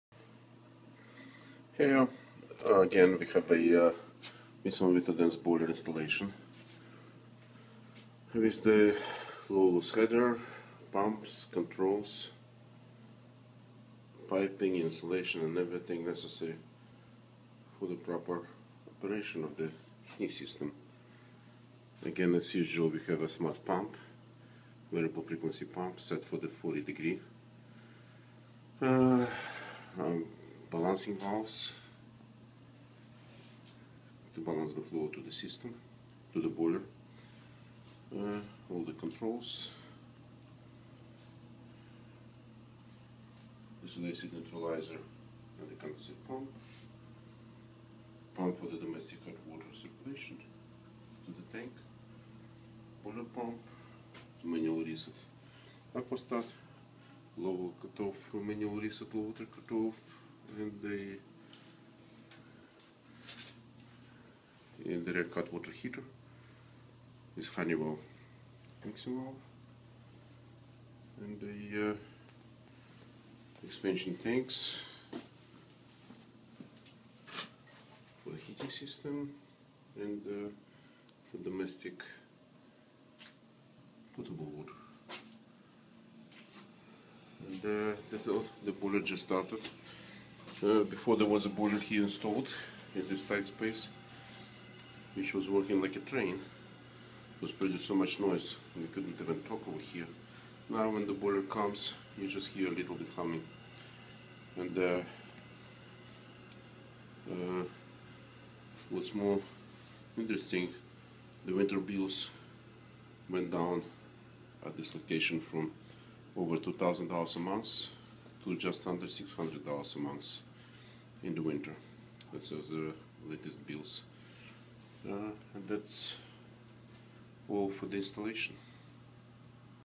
Viessmann vitodens 200WB2B boiler installation
Viessmann vitodens 200WB2B boiler and Weil Mclain indirect water heater Aqua plus installation in upper west side Manhattan, NY.